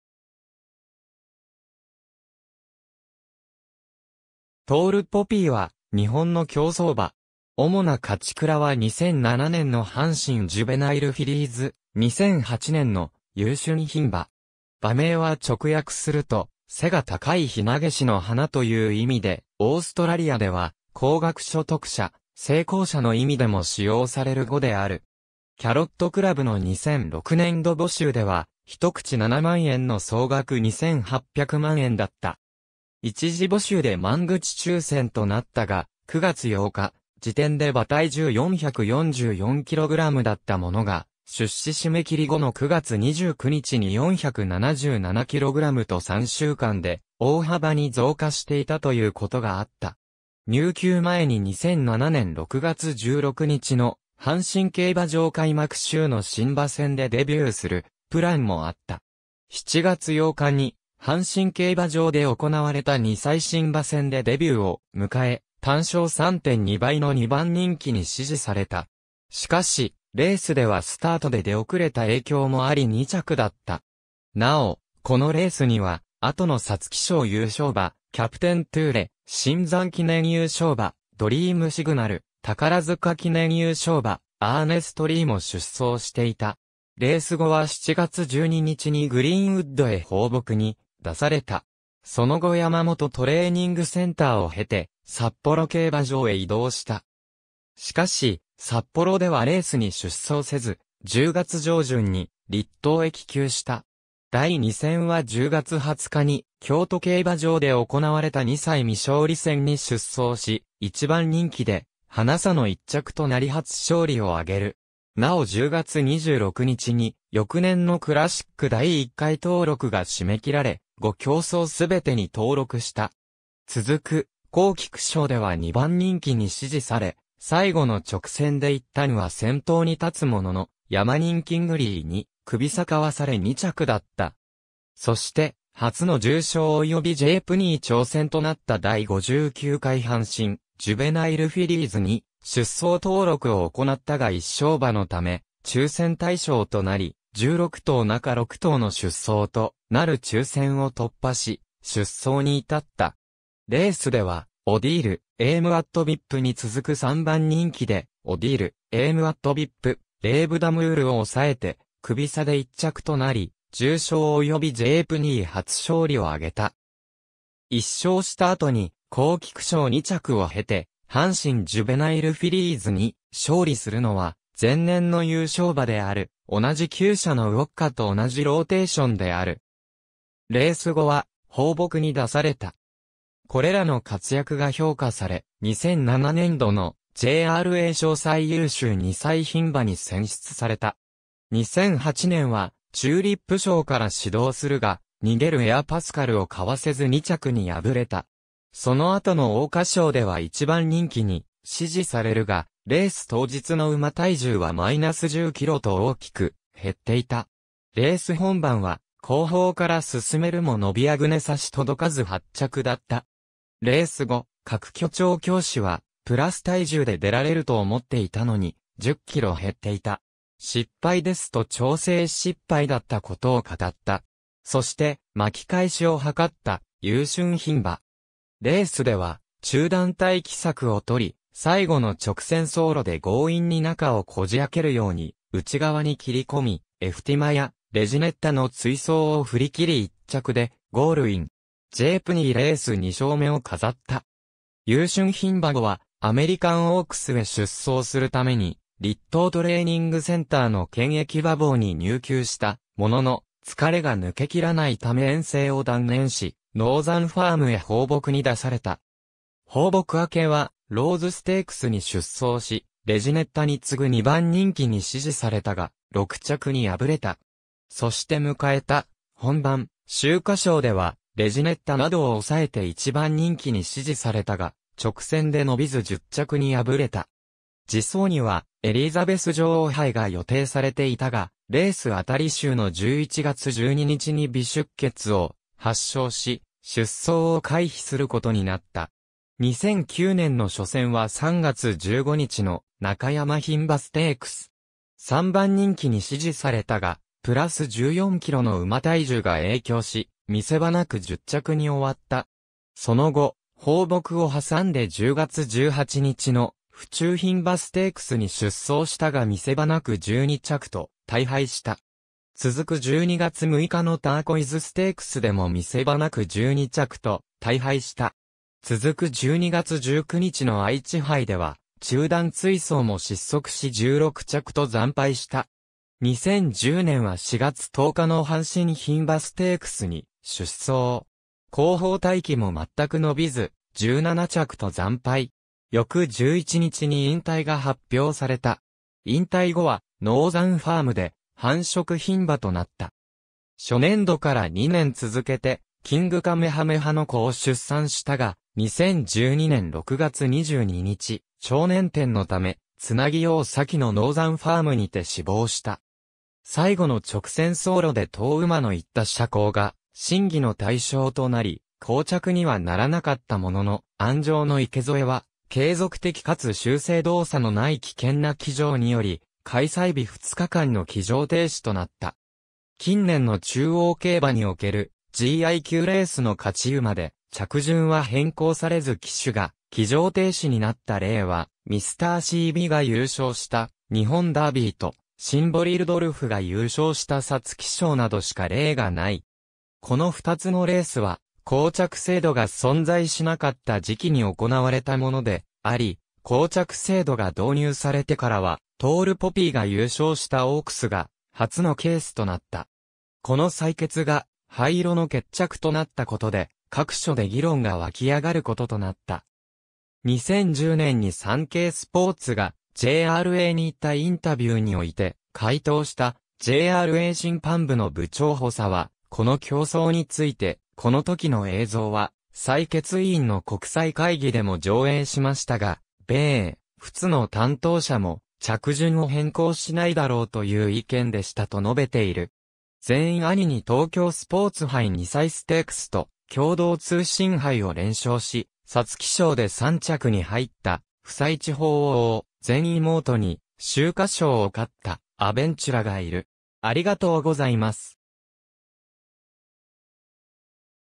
トールポピー
トールポピー (Tall Poppy) は日本の競走馬。
主な勝ち鞍は2007年の阪神ジュベナイルフィリーズ、2008年の優駿牝馬。
馬名は直訳すると「背が高いひなげしの花」という意味で、オーストラリアでは「高額所得者・成功者」の意味でも使用される語である。
キャロットクラブの2006年度募集では一口7万円（全400口）の総額2800万円だった。
一次募集で満口抽選となったが、9月8日時点で馬体重444kgだったものが出資締切後の9月29日に477kgと3週間で大幅に増加していたということがあった。
入厩前に2007年6月16日の阪神競馬場開幕週の新馬戦でデビューするプランもあった。
7月8日に阪神競馬場で行われた2歳新馬戦でデビューを迎え、単勝3.2倍の2番人気に支持された。
しかし、レースではスタートで出遅れた影響もあり2着だった。
なお、このレースには後の皐月賞優勝馬・キャプテントゥーレ、シンザン記念優勝馬・ドリームシグナル、宝塚記念優勝馬・アーネストリーも出走していた。
レース後は7月12日にグリーンウッドへ放牧に出された。
その後山元トレーニングセンターを経て、札幌競馬場へ移動した。
しかし、札幌ではレースに出走せず、10月上旬に栗東へ帰厩した。
第2戦は10月20日に京都競馬場で行われた2歳未勝利戦に出走し、1番人気でハナ差の1着となり初勝利をあげる。
なお10月26日に翌年のクラシック第1回登録が締め切られ、5競走すべてに登録した。
続く黄菊賞（500万下）では2番人気に支持され、最後の直線で一旦は先頭に立つもののヤマニンキングリーにクビ差かわされ2着だった。
そして、初の重賞及びJpnI挑戦となった第59回阪神ジュベナイルフィリーズに出走登録を行ったが1勝馬のため抽選対象となり、16頭中6頭の出走となる抽選を突破し出走に至った。
レースではオディール、エイムアットビップに続く3番人気でオディール、エイムアットビップ、レーヴダムールを抑えてクビ差で1着となり、重賞及びJpnI初勝利を挙げた。
1勝した後に黄菊賞2着を経て阪神ジュベナイルフィリーズに勝利するのは、前年の優勝馬である同じ厩舎のウオッカと同じローテーションである。
レース後は放牧に出された。
これらの活躍が評価され、2007年度のJRA賞最優秀2歳牝馬に選出された。
2008年はチューリップ賞から始動するが、逃げるエアパスカルを交わせず2着に敗れた。
その後の桜花賞では1番人気に支持されるが、レース当日の馬体重は-10キロと大きく減っていた。
レース本番は後方から進めるも伸びあぐね差し届かず8着だった。
レース後、角居調教師は「プラス体重で出られると思っていたのに、10キロ減っていた。失敗です」と調整失敗だったことを語った。
そして巻き返しを図った優駿牝馬（オークス）。
レースでは中段待機策を取り、最後の直線走路で強引に中をこじ開けるように内側に切り込み、エフティマイア、レジネッタの追走を振り切り1着でゴールイン。
JpnIレース2勝目を飾った。
優駿牝馬後はアメリカンオークスへ出走するために栗東トレーニングセンターの検疫馬房に入厩したものの、疲れが抜けきらないため遠征を断念し、ノーザンファームへ放牧に出された。
放牧明けはローズステークスに出走し、レジネッタに次ぐ2番人気に支持されたが、6着に敗れた。
そして迎えた本番、秋華賞ではレジネッタなどを押さえて1番人気に支持されたが、直線で伸びず10着に敗れた。
次走にはエリザベス女王杯が予定されていたが、レース当週の11月12日に鼻出血を発症し出走を回避することになった。
2009年の初戦は3月15日の中山牝馬ステークス。
3番人気に支持されたが、プラス14キロの馬体重が影響し、見せ場なく10着に終わった。
その後、放牧を挟んで10月18日の府中牝馬ステークスに出走したが見せ場なく12着と大敗した。
続く12月6日のターコイズステークスでも見せ場なく12着と大敗した。
続く12月19日の愛知杯では中団追走も失速し16着と惨敗した。
2010年は4月10日の阪神牝馬ステークスに出走。
後方待機もまったく伸びず、17着と惨敗。
翌11日に引退が発表された。
引退後はノーザンファームで繁殖牝馬となった。
初年度から2年続けてキングカメハメハの仔を出産したが、2012年6月22日、腸捻転のため繋養先のノーザンファームにて死亡した。
最後...